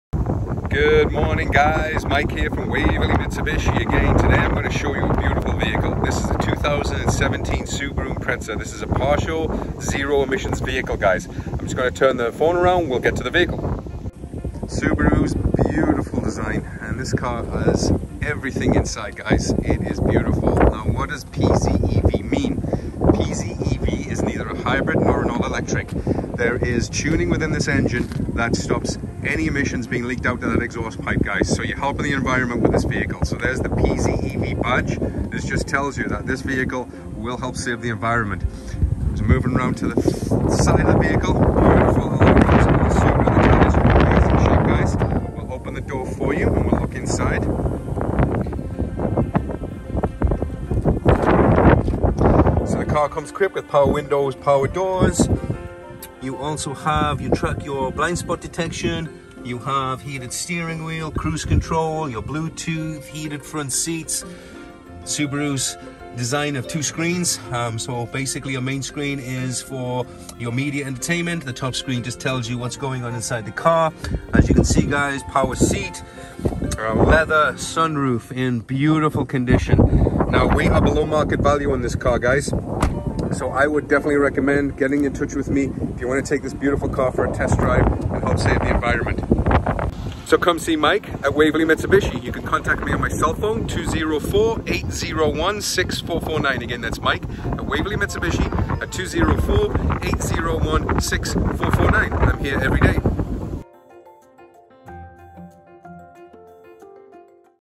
2017 Subaru Impreza @ Waverley Mitsubishi
QUIET, COMFORTABLE AND ROOMY CABIN, STANDARD AWD, LONG LIST OF SAFETY FEATURES. IT``S A PARTIAL HYBRID VEHICLE!

ONLY HERE @ WAVERLEY MITSUBISHI! $500 Amazon gift card cannot be combined with other promotions or discounts.